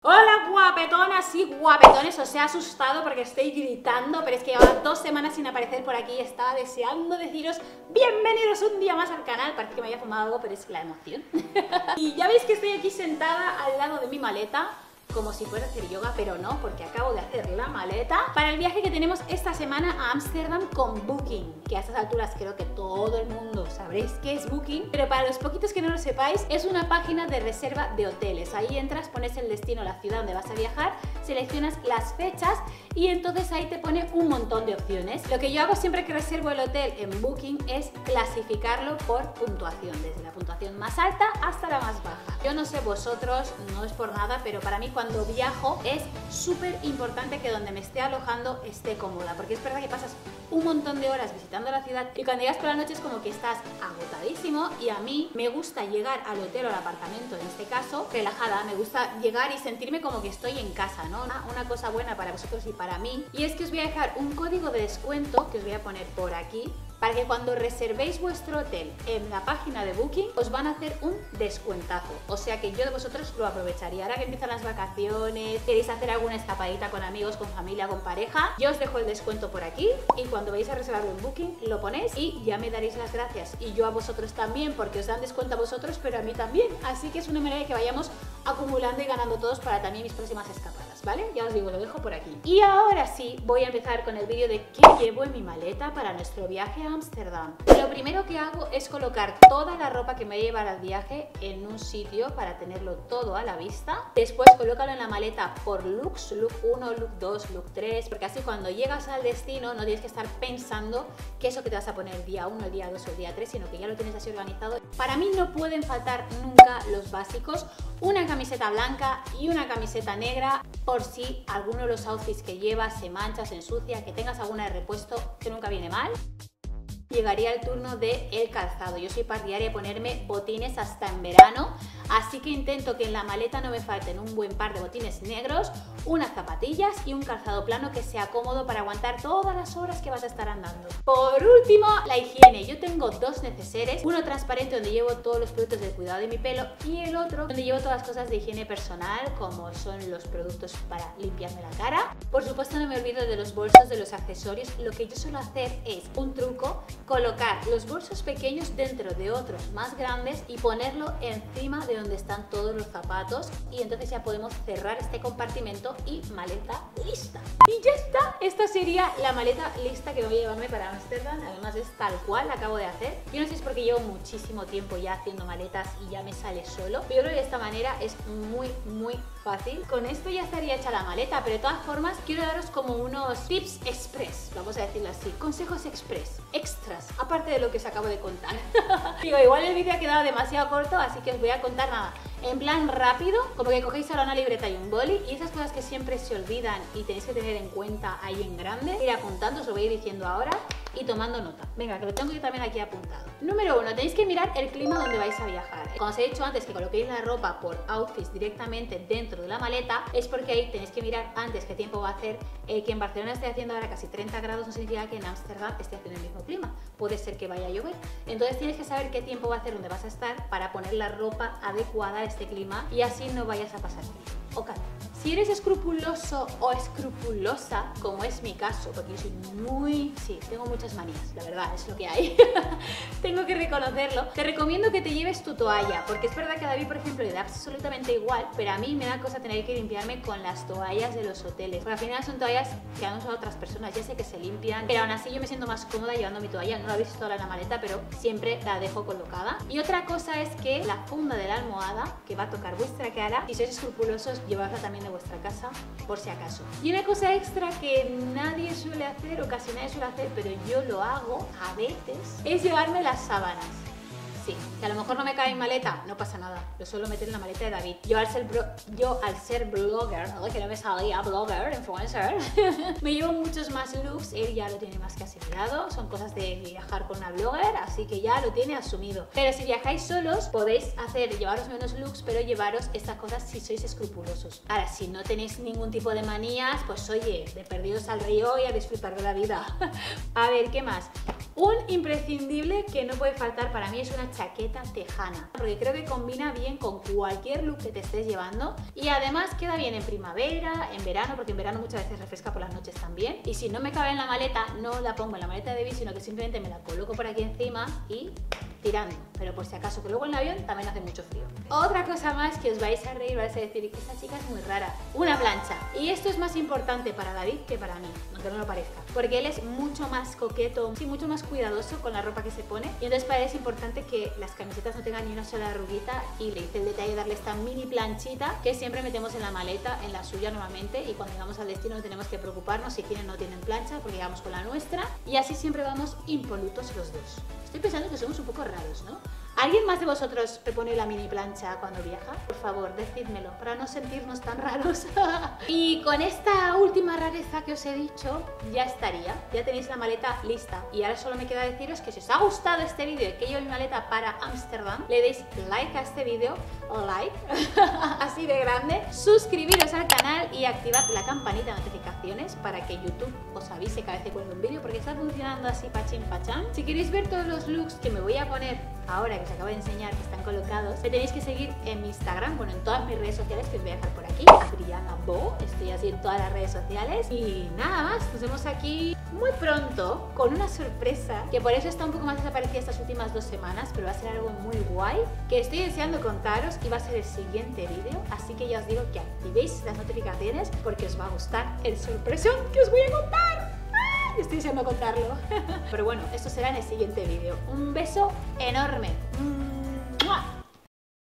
Cómo hago mi maleta para Ámsterdam| ADRIANA BOHO
Qué llevo en mi maleta para nuestro viaje a Ámsterdam? LINK PARA EL 15% DE